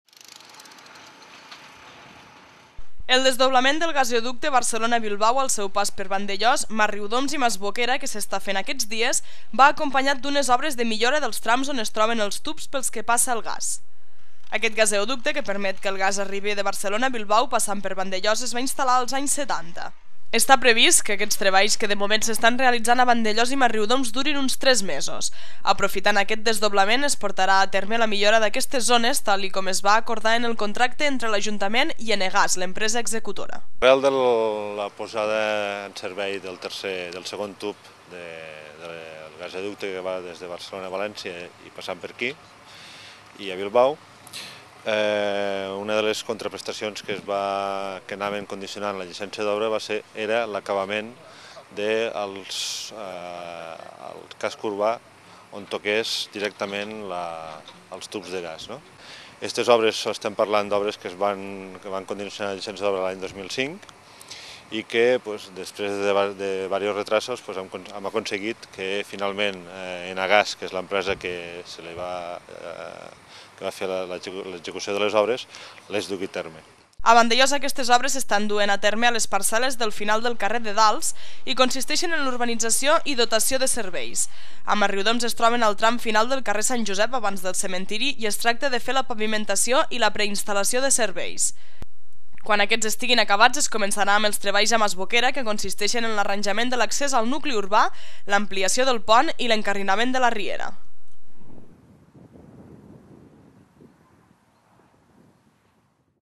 Desdoblament del gasoducte Barcelona-Bilbao
Aquest gasoducte que permet que el gas arribi de Barcelona a Bilbao passant per Vandellòs es va instal•lar als anys 70. Està previst que aquests treballs, que de moment sestan realitzant a Vandellòs i Masriudoms durin uns 3 mesos.